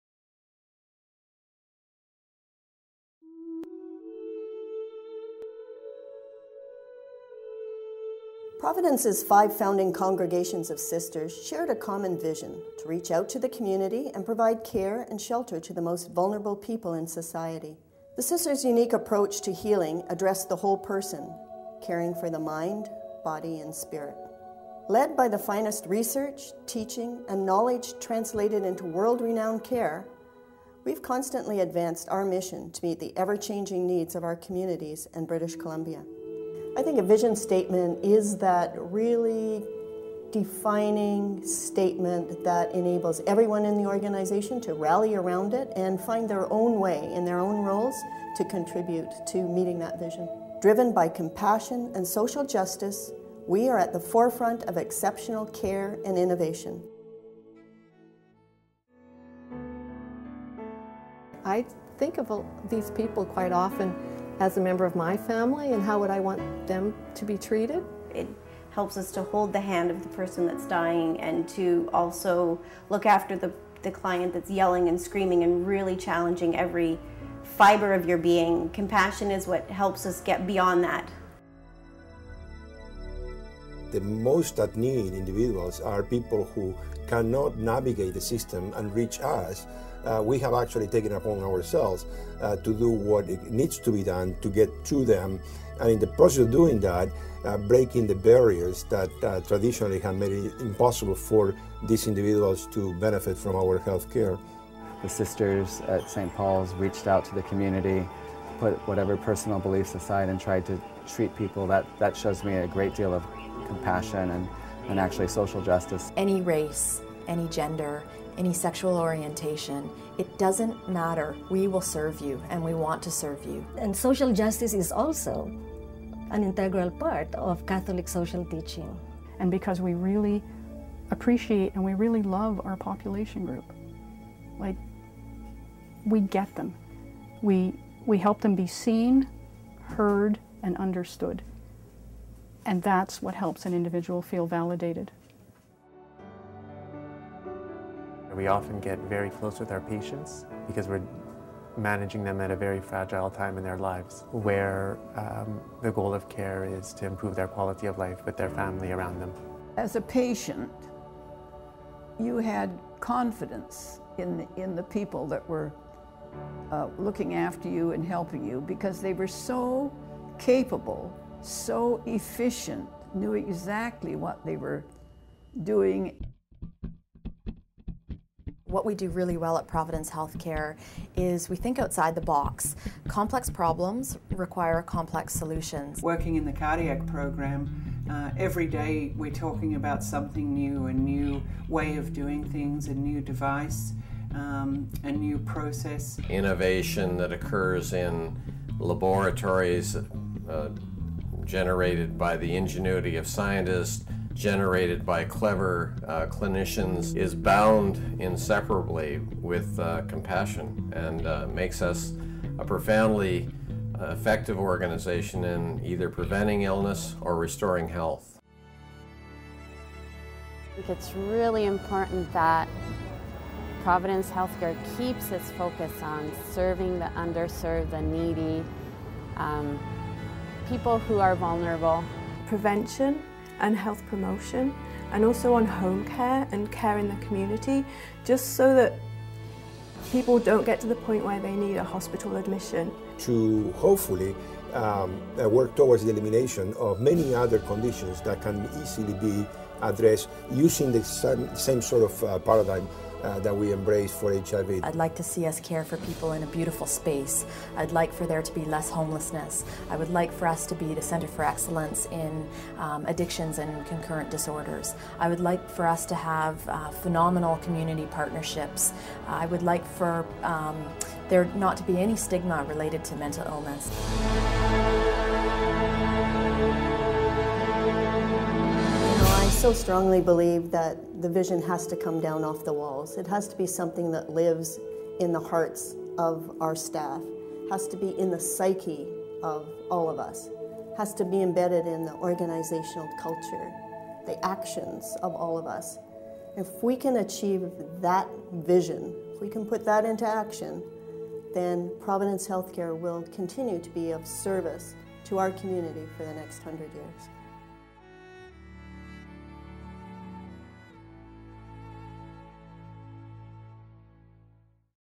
PHC Vision Statement (short version)
New Vision Statement Reflective of PHC's Growth and Future Path

Providence Health Care has a new Vision statement -- one that builds on our rich 118-year history of compassionate care, and looks to the future with boldness, inspiration and urgent desire to improve the lives of British Columbians.

The new Vision states: "Driven by compassion and social justice, we are at the forefront of exceptional care and innovation."

It replaces the older Vision statement, which had stated: "We will continue to grow as a community, regional and academic health science enterprise that is a recognized leader, and major player, in the provision of health care within British Columbia.  We will be respected for our care and services, known for our Mission and Values, acknowledged for the contributions of our teachers and researchers.  We will actualize our Vision by being an organization of "caring hearts, creative souls, and resourceful actions."

Dianne Doyle, President & CEO, says the new Vision was required to reflect PHC's evolving strategic aspirations and goals for continuing to make leadership contributions in health care delivery, teaching and research.

"To me the new Vision acknowledges and symbolizes the excellence and deep commitment each of our staff, caregivers, physicians and researchers have to patients, residents, communities and to enhancing human knowledge," says Doyle. "It's saying 'This is who we are and where we come from -- a strong values- and faith-based organization.  This is what we believe in -- caring and advocating for society's most vulnerable populations. And this is what we're committed to, and where we'll be when we've reached our destination -- to the forefront of exceptional care and innovation.'"